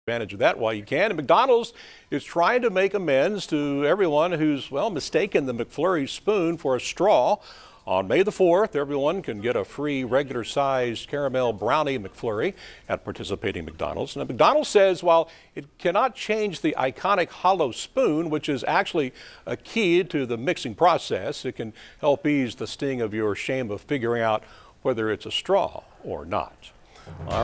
McDonald's offering free caramel brownie McFlurrys on May 4
McDonald's is trying to make amends to everyone who's mistaken the McFlurry spoon for a straw.